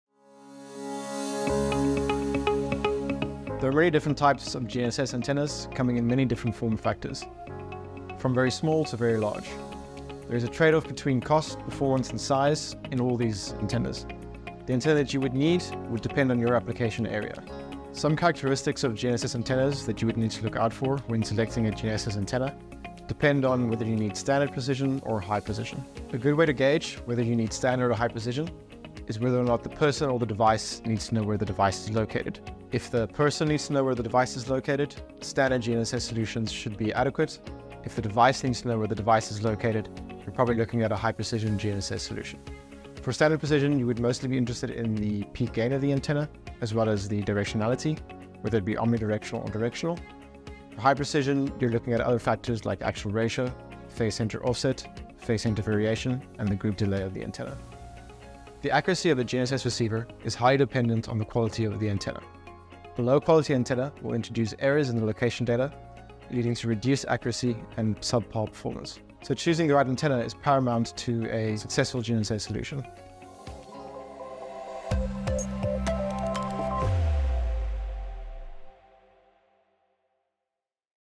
Choosing the Right GNSS Antenna | What to Consider
Selecting the right GNSS antenna is a critical decision that shapes the accuracy and efficiency of your entire system.

Hear from Lukas Van Vuuren, Senior Antenna Engineer at Taoglas, who explains the key considerations when choosing the right GNSS antenna for your application.

For more in-depth information, visit our GNSS Essential Knowledge